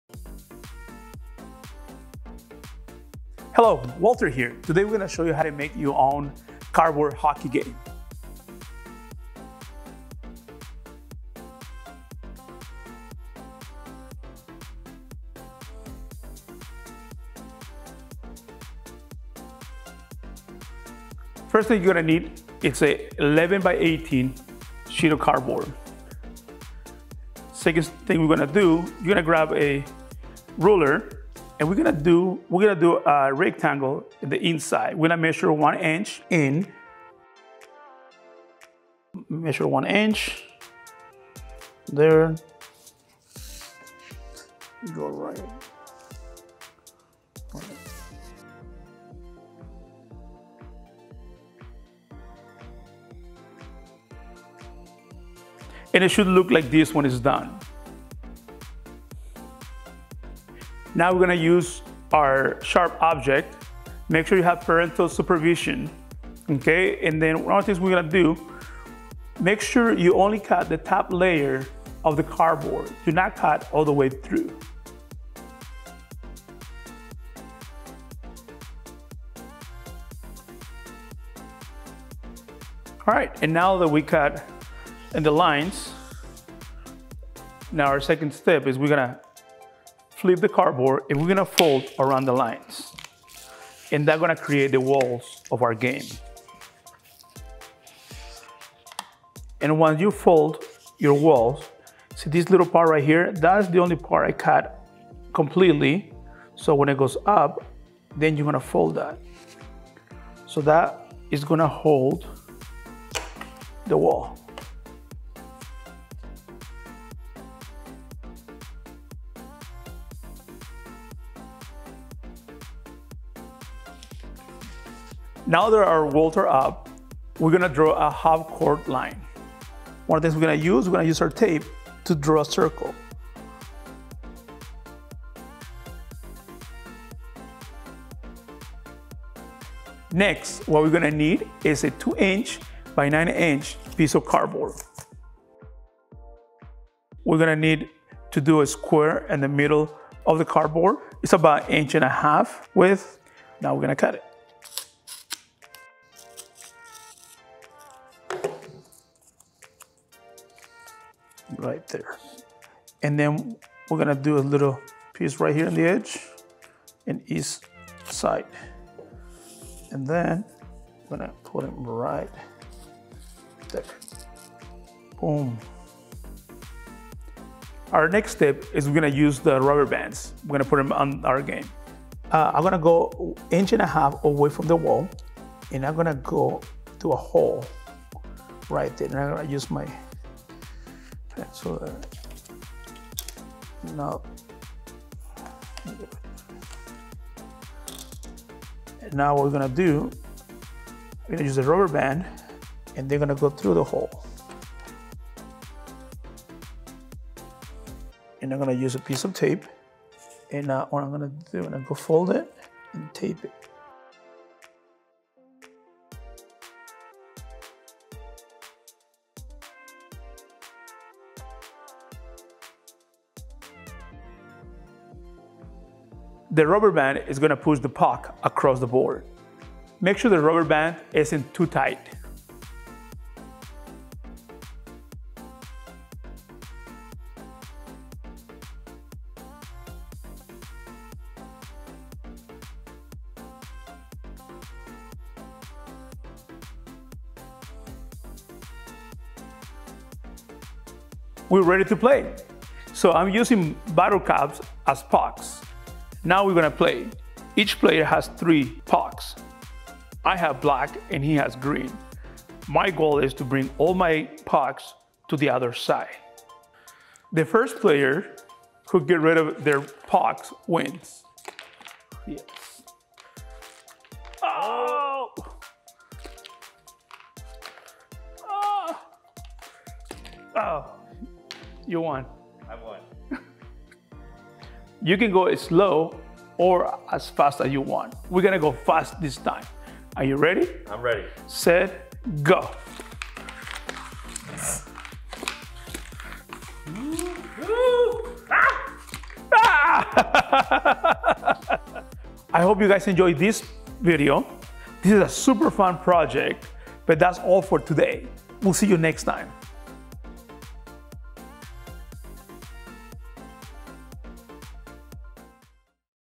DIY Project: How To Make A Cardboard Hockey Game
Calling all creators! Looking for an exciting way to give some extra cardboard scraps a second life? Check out this DIY project!

In this video, Walter shows you how to use a few household items to create a fast-paced Cardboard Hockey Game!

This is a great hands-on project to do at home, and you can customize the game however you like. You can make the hockey board as large or as small as you want depending on how much cardboard you have, and you can build your center divider using one or multiple spaces - make it your own! After you've put your game together, grab a friend, make up your own rules, and start playing!

Materials needed:
- Pieces of cardboard
- Rubber bands
- Ruler
- Box cutter
- Bottle caps
- Tape

When you’re done with this project, we’d love to see it!

Have fun!

A Virtual Club video from the Boys & Girls Clubs of Benton and Franklin Counties.